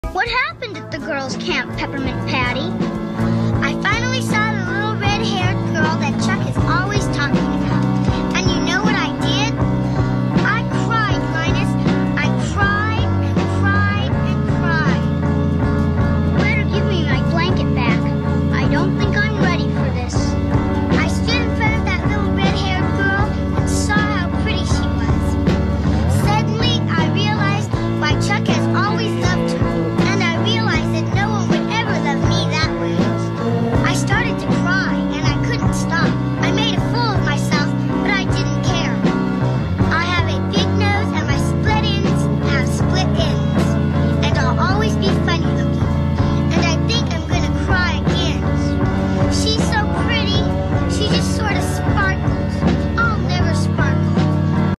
She just sorta sparkles :
I had this edit in my mind for months. Just like applied to the Makani, Kalia, Amaya situation. Now, it seems as if I’m K.

Why do I let you put me through all of this? Why do I have mascara stains on my sheets? Why did I spend $50 today for us to have a nice movie date for you to bring up the girl from your past beforehand. Or the time you processed your thoughts for 3 nights, and I was left to sob instead.

And how you said; “I don’t think our relationship is the best if were being honest.”

I try so hard to be better than your exes. In the end, do I fall short?
do I deserve better…?

Credits;
Orcinus footages
Forevershouka footages